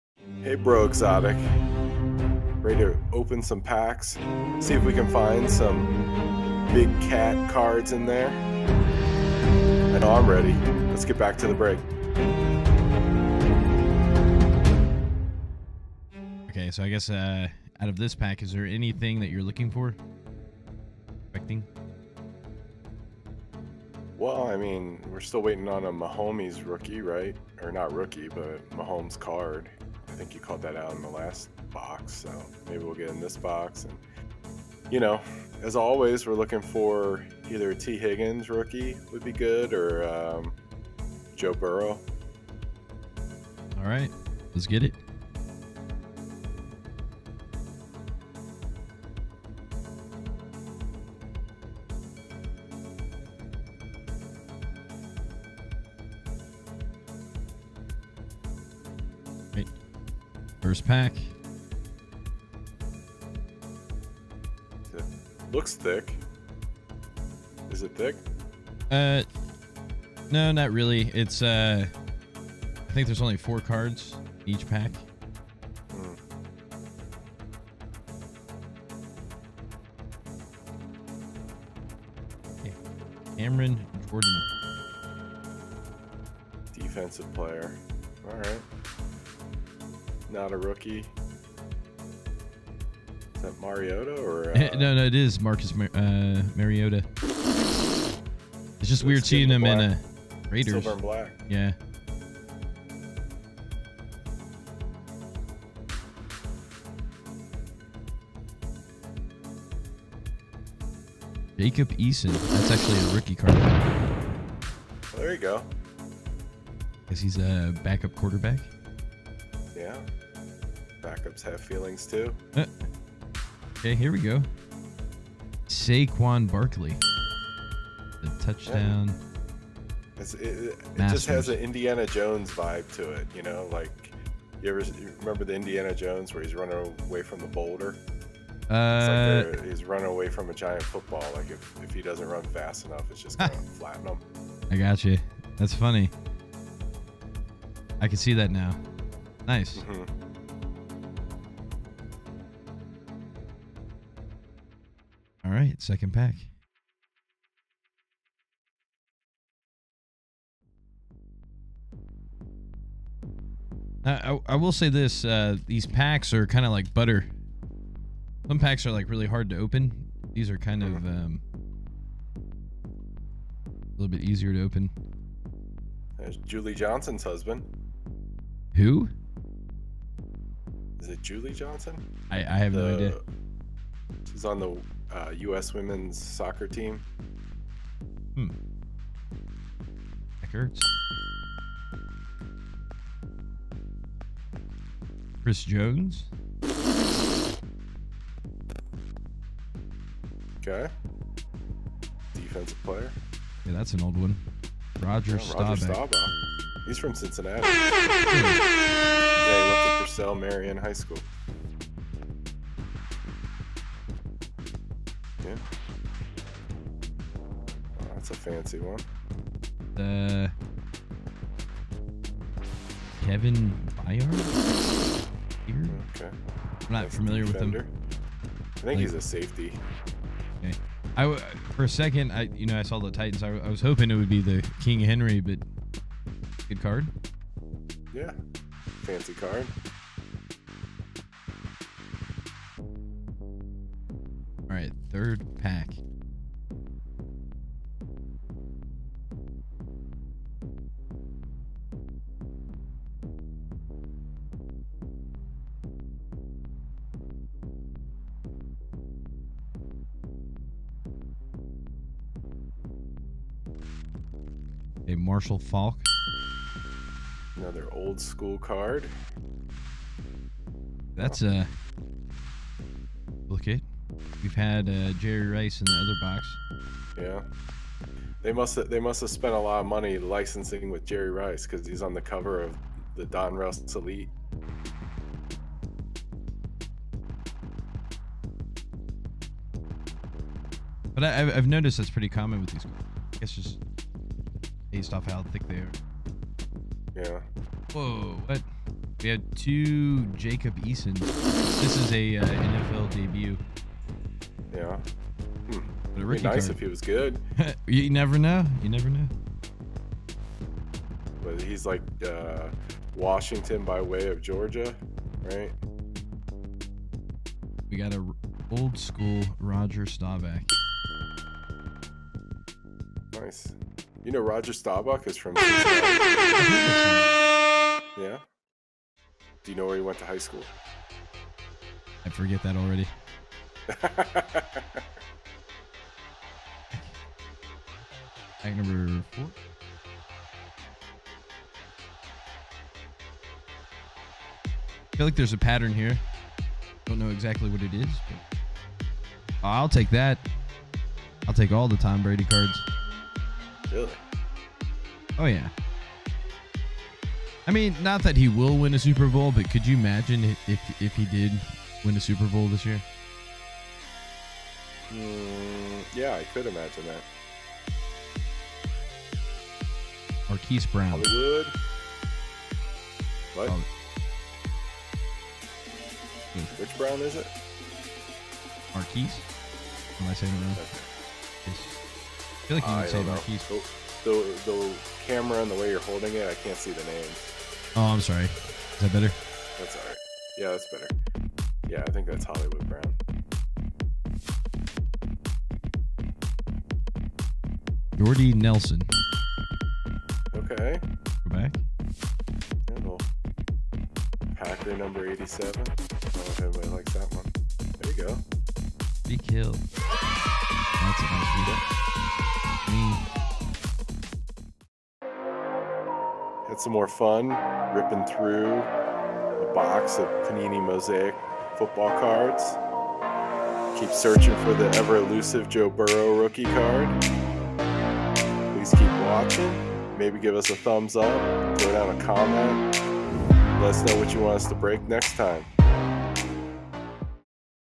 Break Broz 2020 NFL Mosaic EP 3
Breaker Broz open their second blaster box of 2020 Panini Mosaic!